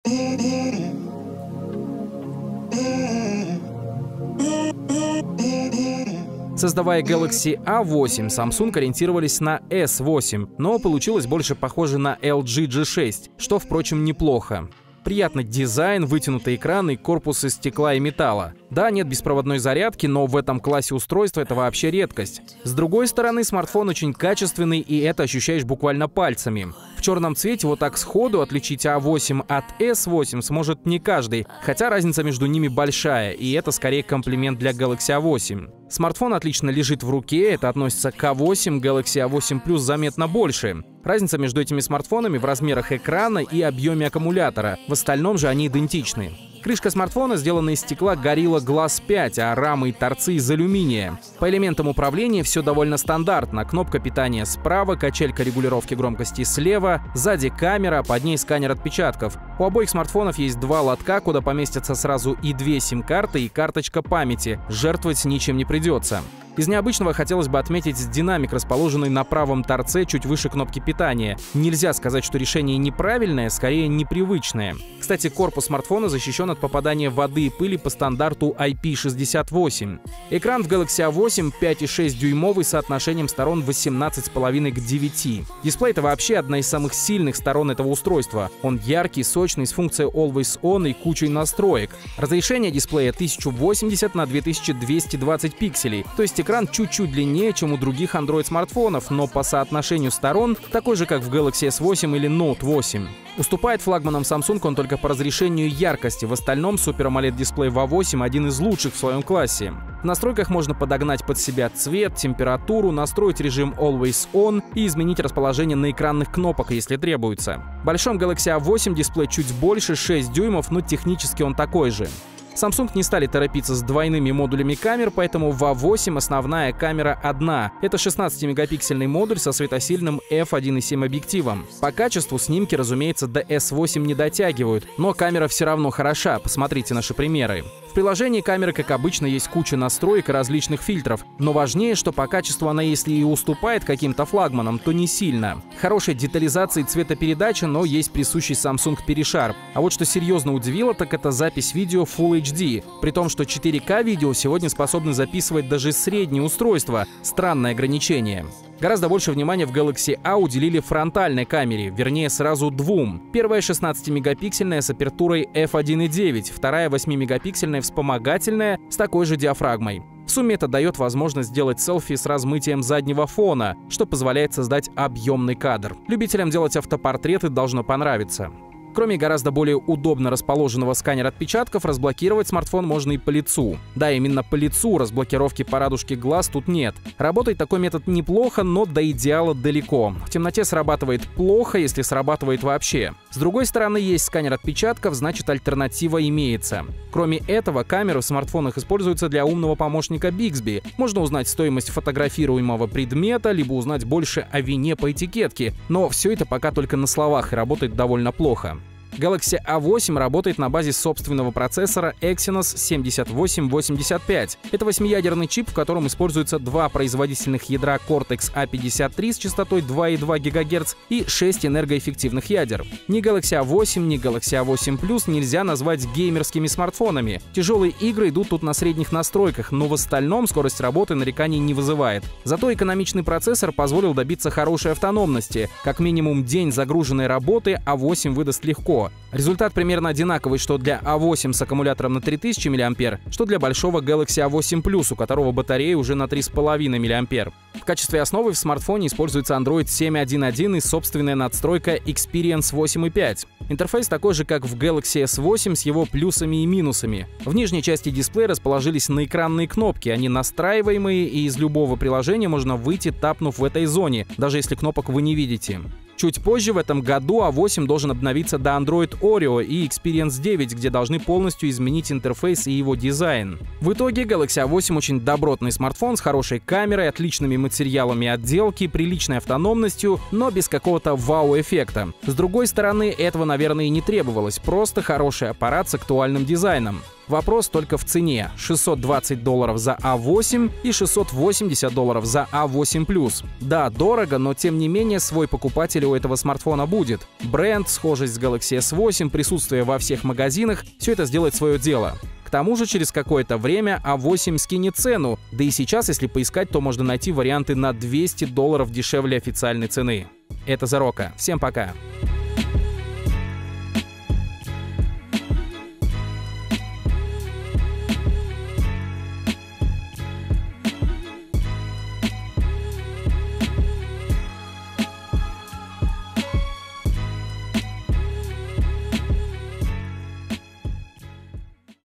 Обзор Samsung Galaxy A8 - почти флагман...
Почти флагман. Почти Galaxy S8. Почти безрамочный. Samsung Galaxy A8 претендует на лавры дорогих флагманских смартфонов. Хотя и сам Galaxy A8 дешевым язык назвать не повернется, но новинка однозначно интересная и составит конкуренцию китайским флагманам.

И кстати, ставь Like и мы будем знать, какие обзоры и сюжеты тебе нравятся больше. Впереди много очень крутых обзоров и новостей, не пропусти!

В обзоре использована музыка